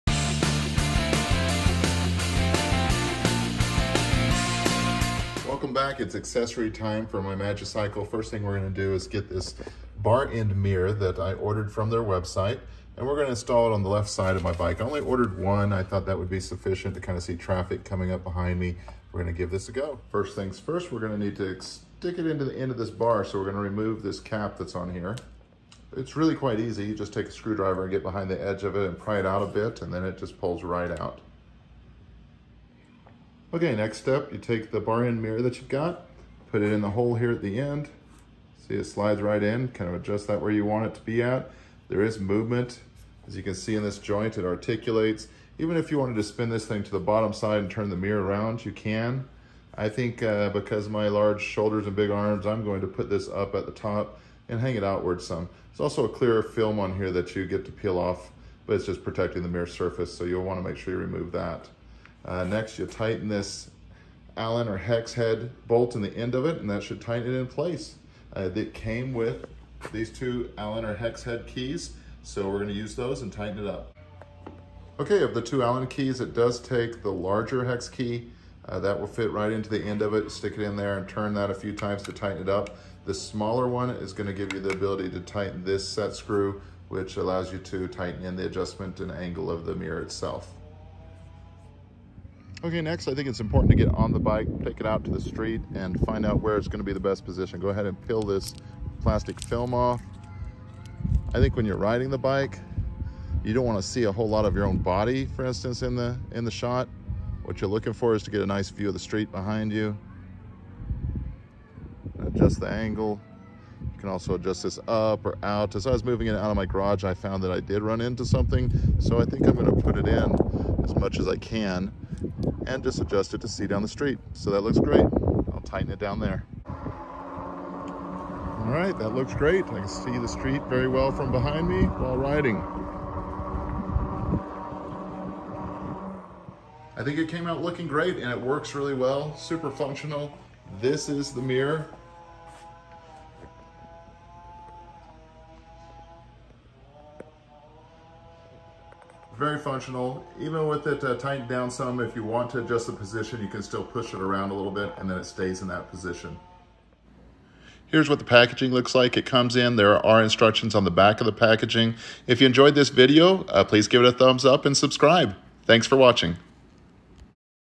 Magicycle Bar End Mirror installation. E-bike Mirror
Here we go through the installation of the bar end mirror accessory available from the Magicycle bike website. This is a great accessory and easy to install and very affordable as well  ￼